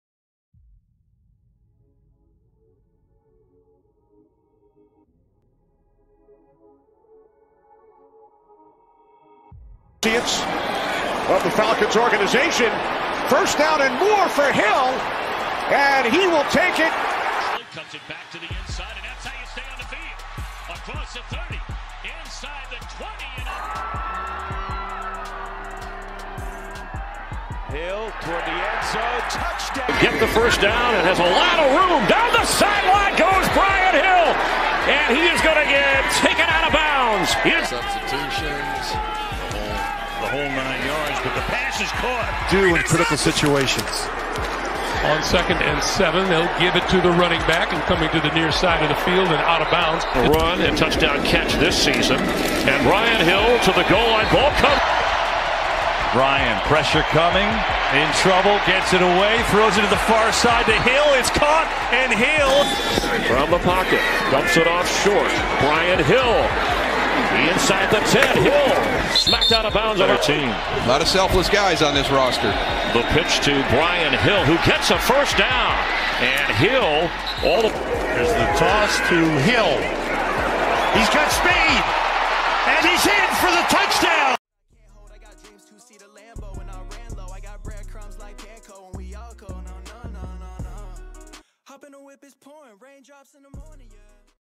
SAN FRANCISCO 49ERS: Brian Hill ᴴᴰ
BREAKING NEWS || SAN FRANCISCO 49ERS || BRIAN HILL HIGHLIGHTS
44 CAREER GAMES
209 ATTEMPTS
982 YARDS
3 TOUCHDOWNS